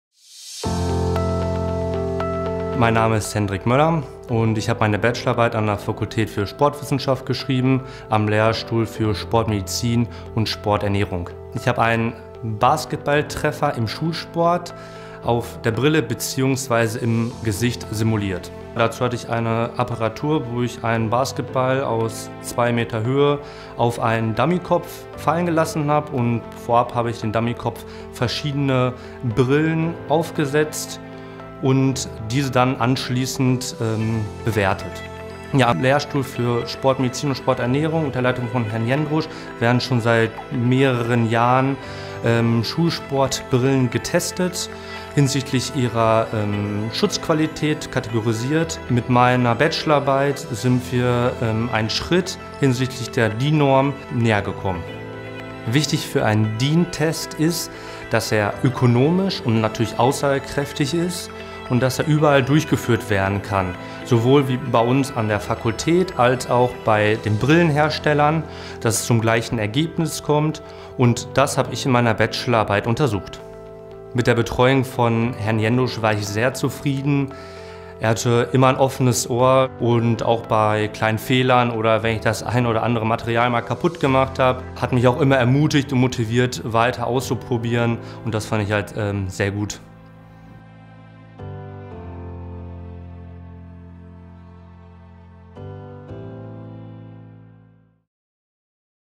Ausgezeichnete Abschlussarbeit 2020: Hendrik Möller im Interview
Hendrik Möller ist Sportwissenschaftler und hat für seine Bachelorarbeit Basketbälle auf Brillen geworfen. Damit seine Ergebnisse vergleichbar und aussagekräftig sind und für DIN-Normen genutzt werden können, hat er für die Ballwurfsimulation eine Apparatur eingesetzt.

Seine Arbeit „Basketball-Fallversuch zur Simulation eines ‚Ball-Treffers‘ im Schulsport auf der Brille bzw. im Gesicht – Erprobung und Evaluation eines neuen Testverfahrens“ soll dabei helfen, Schulsportbrillen zu verbessern. Dafür wird Hendrik bei der digitalen RUBjahresfeier ausgezeichnet.